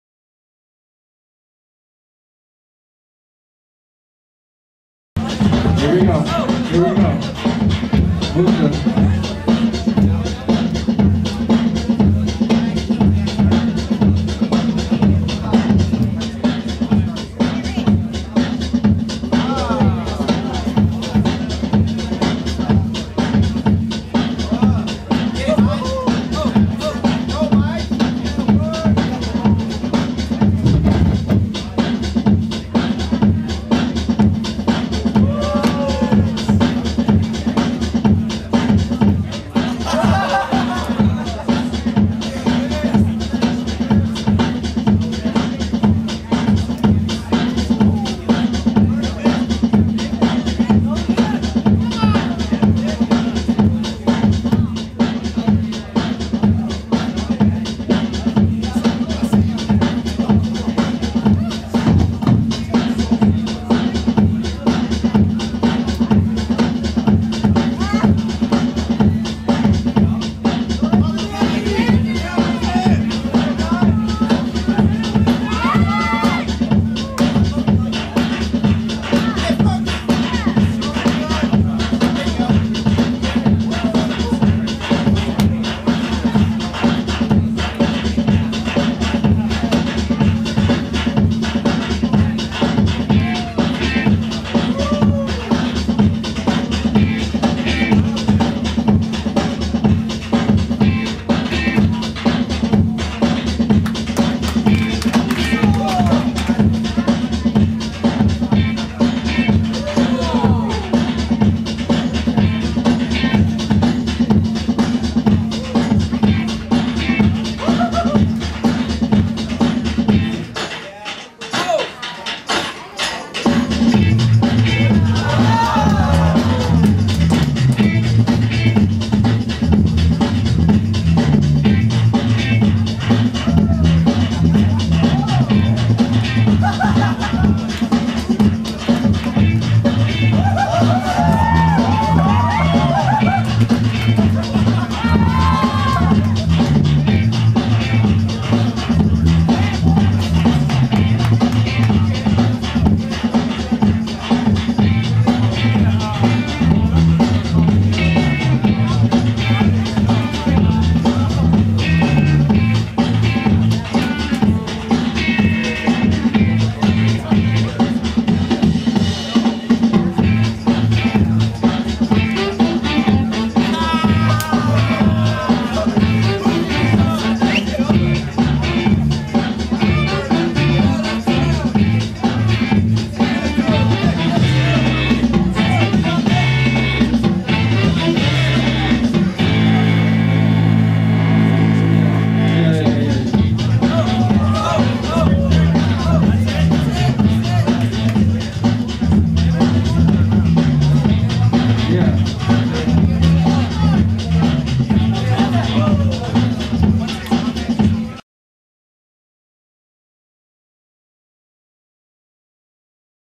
Toprock battle at the Beatdown Session in Denver
Hip-hop Congress presents the Beatdown session footage. Sorry if we didn't get everyone